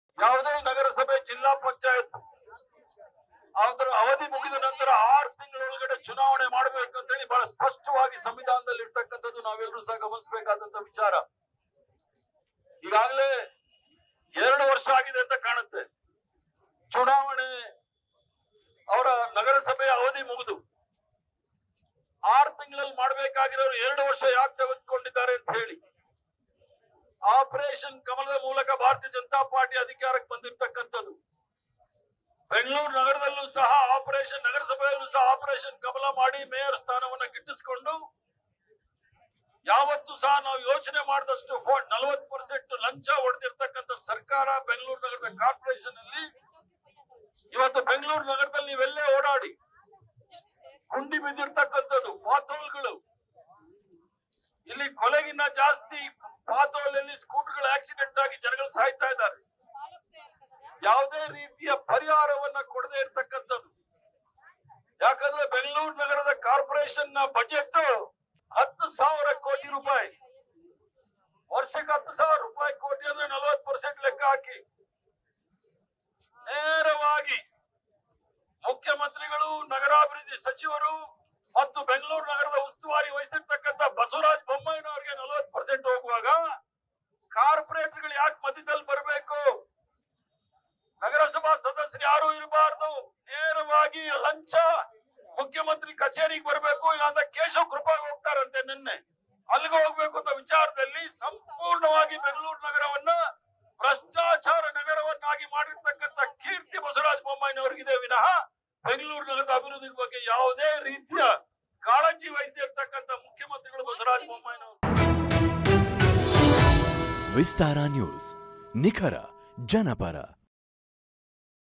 B K Hariprasad : ಬೊಮ್ಮಾಯಿಗೆ 40% ಹೋಗುವಾಗ ಕಾರ್ಪೋರೆಟರ್​ಗಳು ಯಾಕೆ? | Vistara News Kannada | CM Bommai
ಬಿಬಿಎಂಪಿ ವಾರ್ಡ್ ಮೀಸಲಾತಿ ಮರುವಿಂಗಡಣೆ ವಿರೋಧಿಸಿ ಬೆಂಗಳೂರಿನಲ್ಲಿ ಕಾಂಗ್ರೆಸ್ ಹಮ್ಮಿಕೊಂಡಿರುವ ಪ್ರತಿಭಟನೆಯಲ್ಲಿ ಕಾಂಗ್ರೆಸ್ ವಿಧಾನ ಪರಿಷತ್ ನಾಯಕ ಬಿ. ಕೆ. ಹರಿಪ್ರಸಾದ್ ಮಾತನಾಡಿದ್ದಾರೆ.